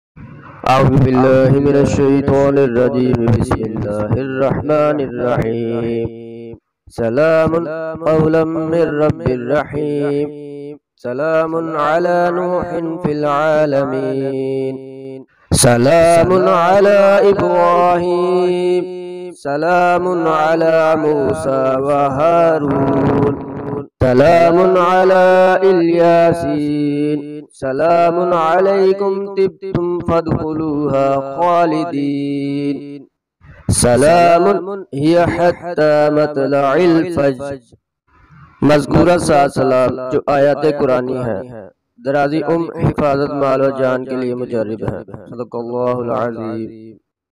7Salam in Quran | salamty ke ley Dua ( Lambi umer our Hifazat ke ley
Al-Quran7999
سات سلام آیات
Ayat e salam in Quran salamti long life

This video use education purpose

Muslims education

HD Arabic text video with urdu translation text and voice